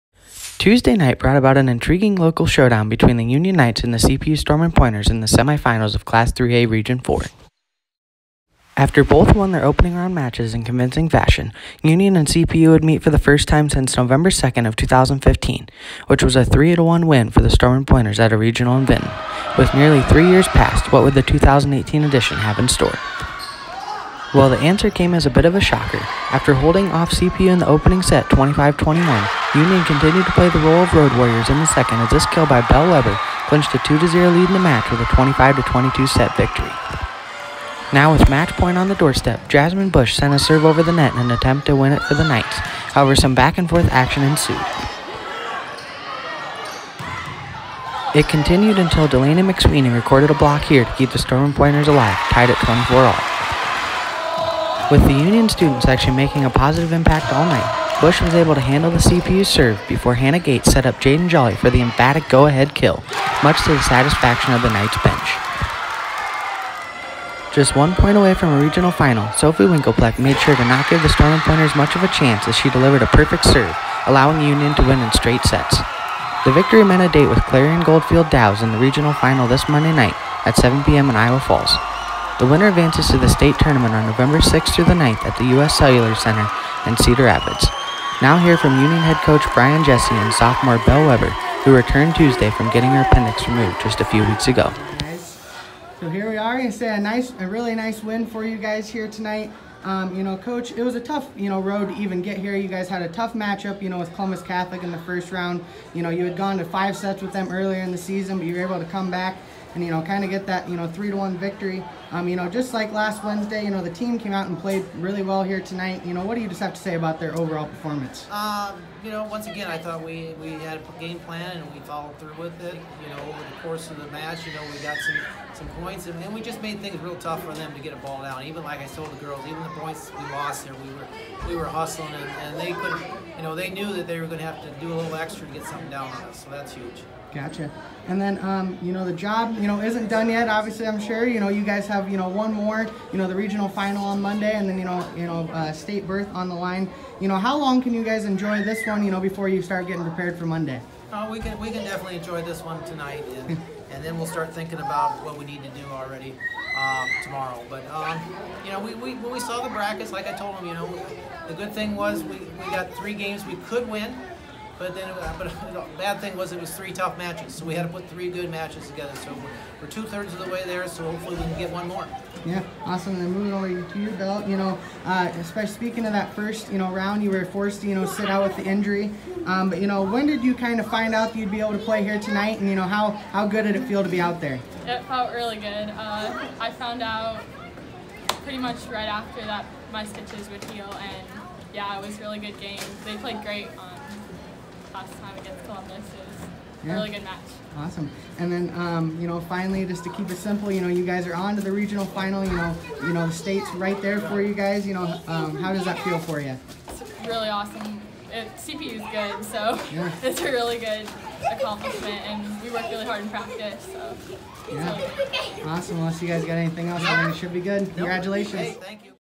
Volleyball highlights: Union at CPU
See video highlights, along with an interview  from Union’s Belle Weber and Head Coach Brian Jesse, from the Knights’ straight set upset victory over the No. 5 CPU Stormin’ Pointers in the Class 3A Region 4 Semi-Final on October 23, 2018.